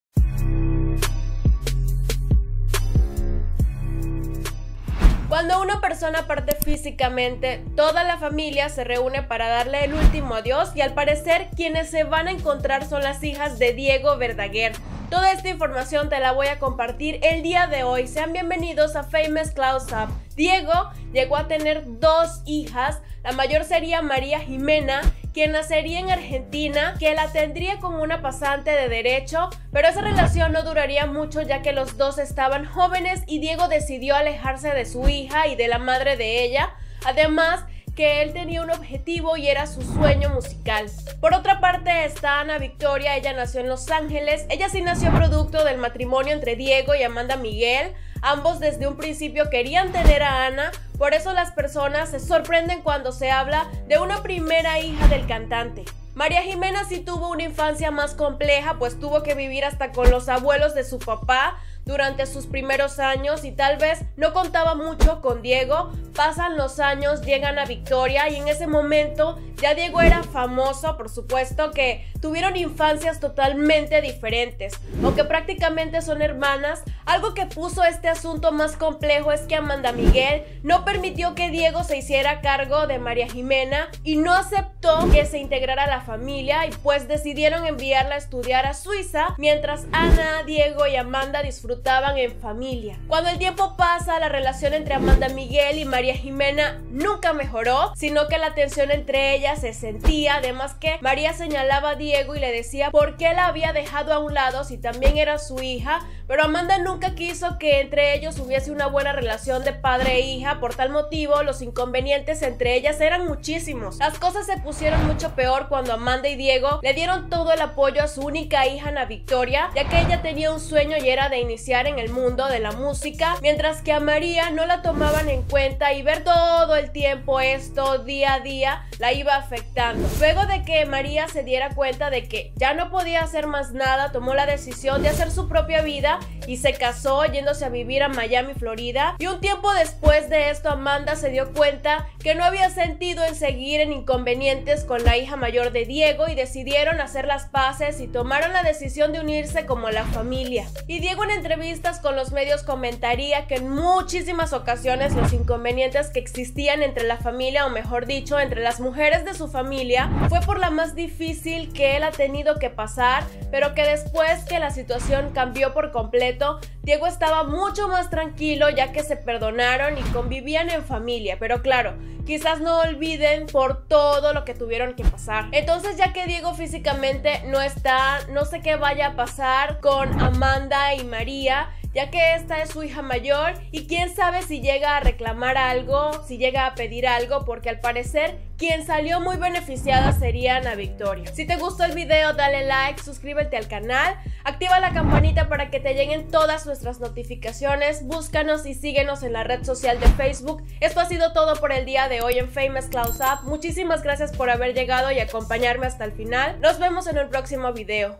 ✝ 𝗧𝗥𝗔𝗦 𝗦𝗨 𝗣𝗔𝗥𝗧𝗜𝗥 hijas de Diego Verdaguer se verán las caras 😱
Se perdonaron, pero nunca olvidaron…